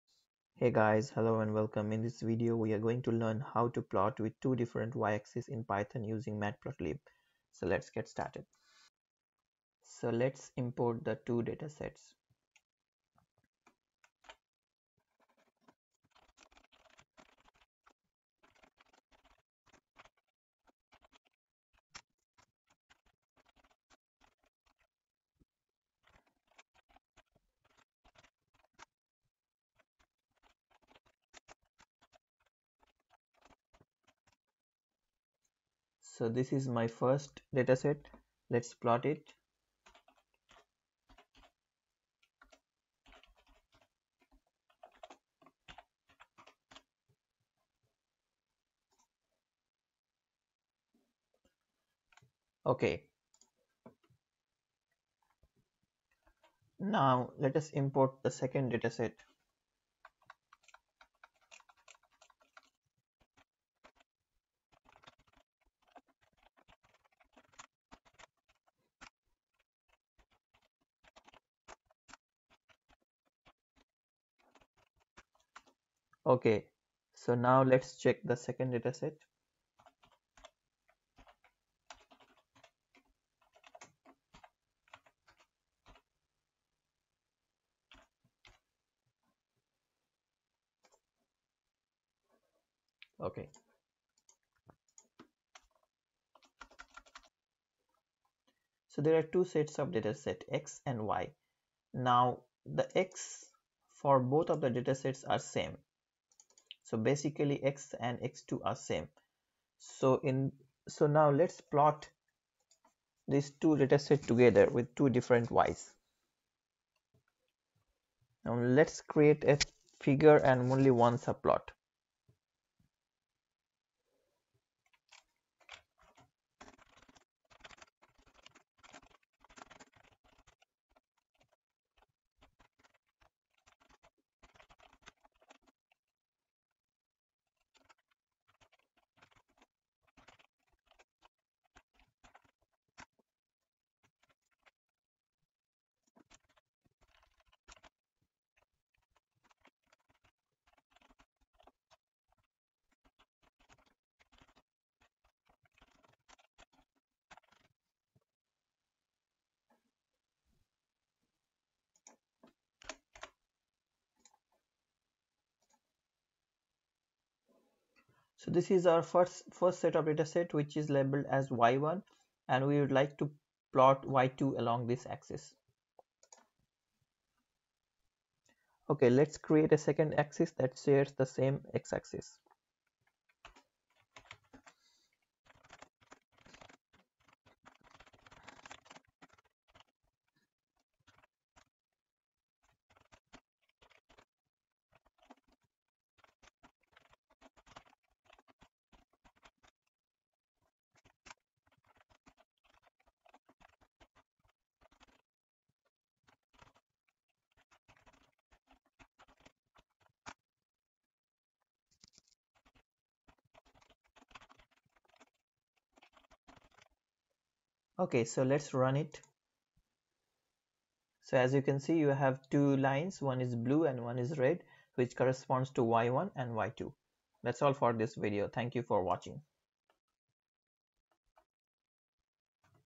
Python plotting with matplotlib (Part 2): Make a Plot with Two Different Y-axis in Python
In this video we are going to learn how to make a plot with two different Y-axis in python with Matplotlib. First, import your first data, then create just a figure and only one subplot and then create a second axes that shares the same x axis, and plot.